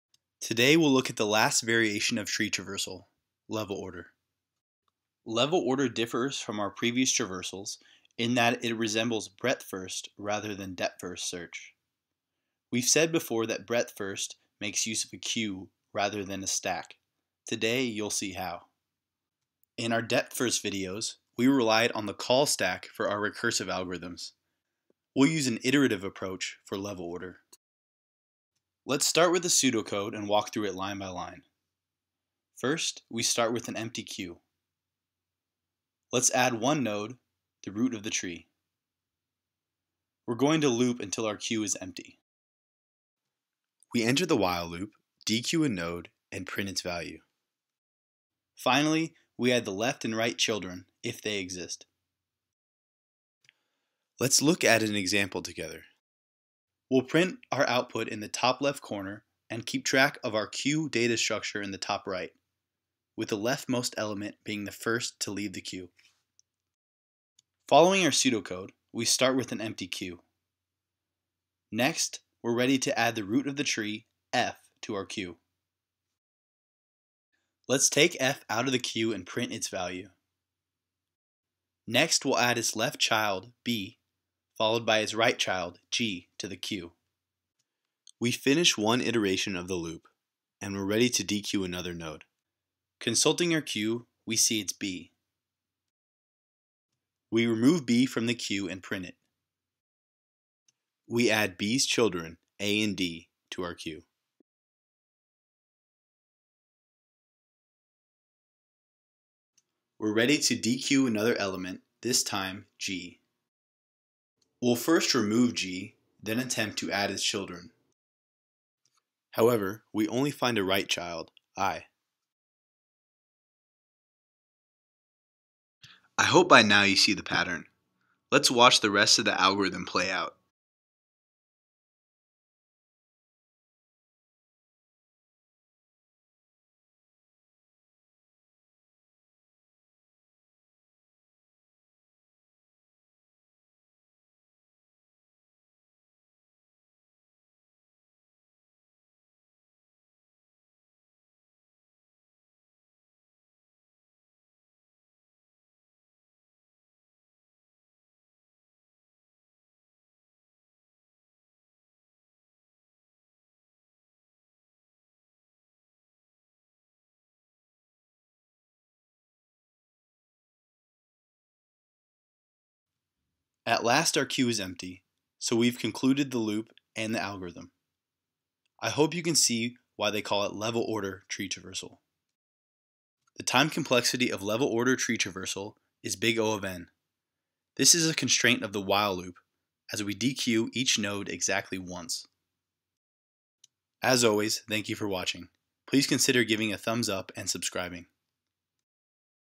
Level-order tree traversal in 3 minutes
Step by step instructions showing how to do level-order tree traversal on a binary tree.